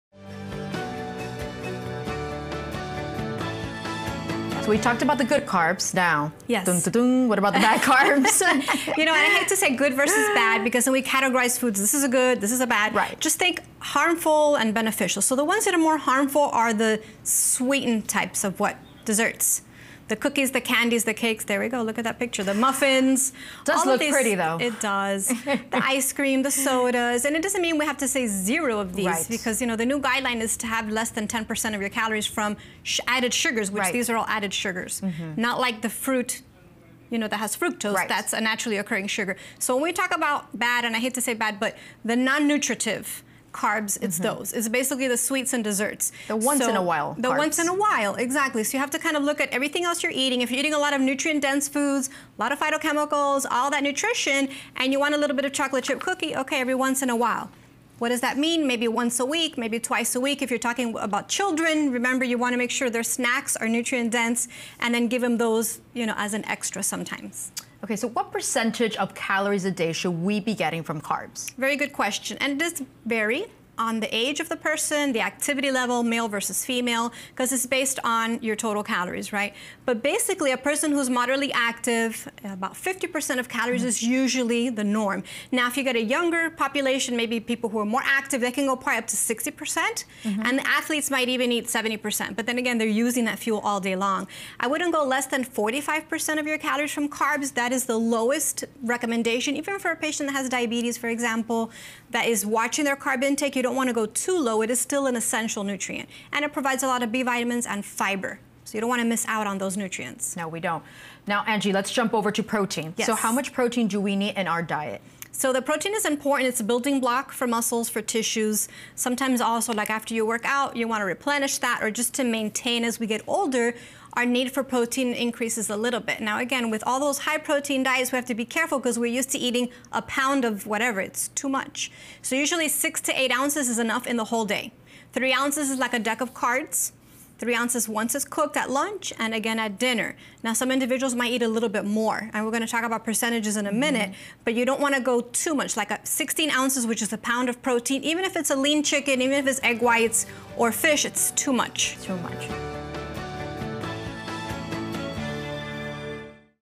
Beneficial vs. Harmful Carbs
Angie Placeres, Registered Dietitian with Baptist Health South Florida, explains sweetened types of carbs, such as desserts, are the most harmful carbs. She affirms the new guidelines say that people have less than 10% of their calories from added sugars and 50% of daily calories from carbs is usually the norm for a younger population and active people.
Protein is important for building muscles and tissues. Six to eight ounces of protein are enough in the whole day, the expert says.